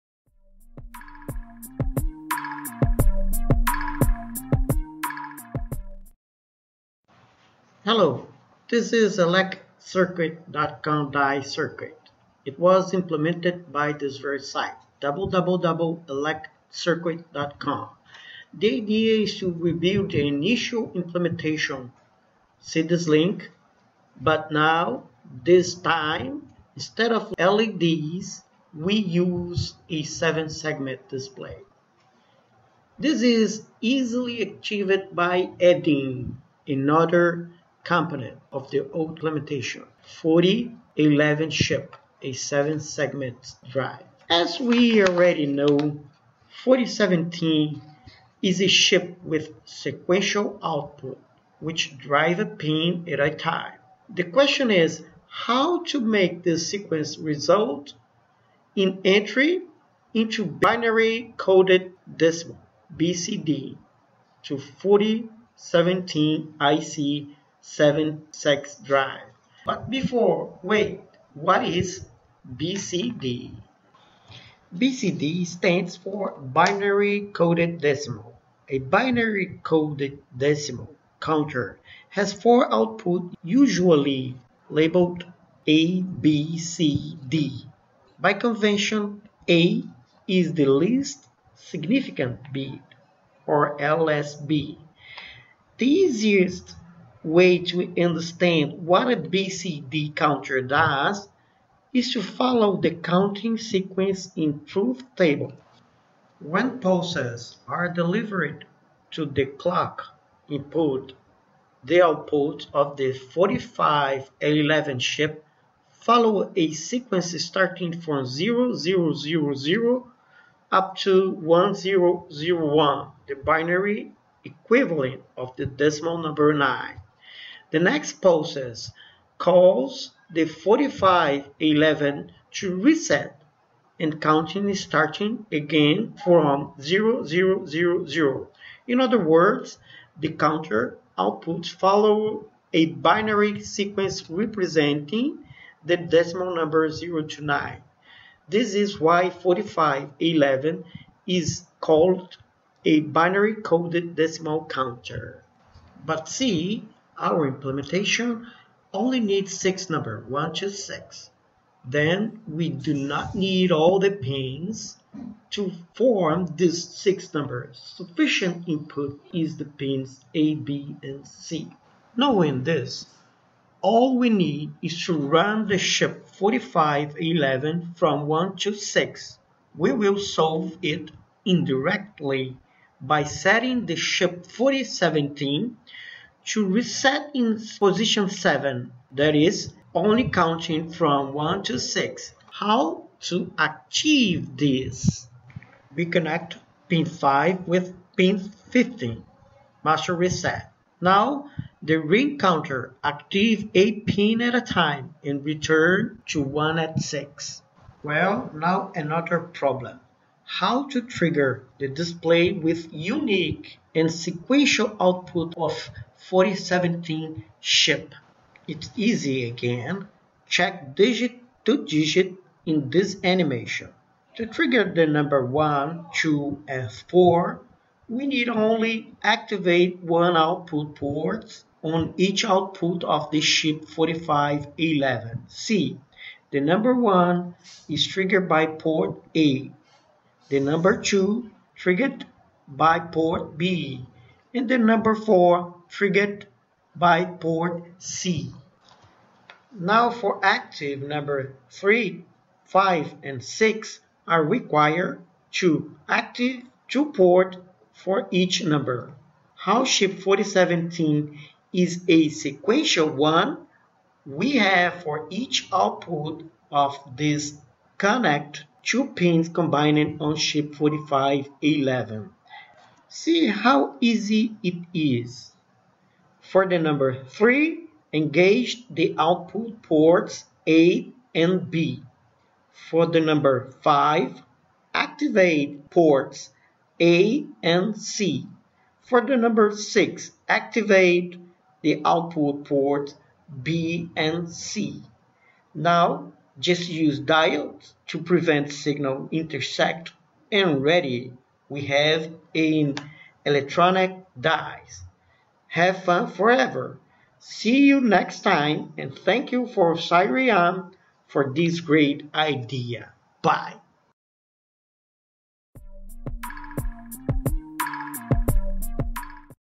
Proteus - IC 4017 + IC 4511 - ElecCircuit Dot Com Dice!
This is ELECCIRCUIT.COM DICE Circuit; It was implemented by this very The idea is to rebuild an initial implementation, see this . But now, this time, instead of leds, we will use a 7 segments display.